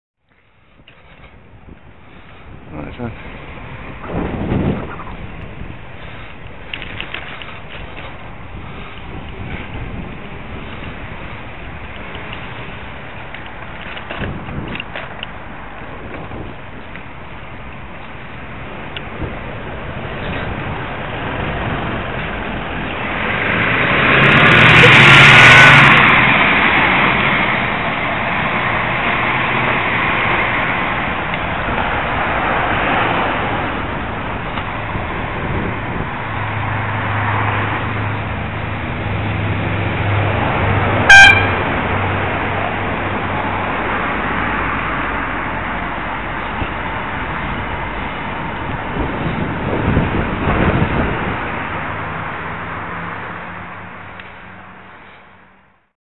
York Urban Cyclist. Leeman Road.
A thank you wave after I pulled in to let a Park & Ride bus driver overtake me. followed by a demonstration of what happens if a cyclist skulks in the kerb-hogging secondary position, on a narrow section of roadway.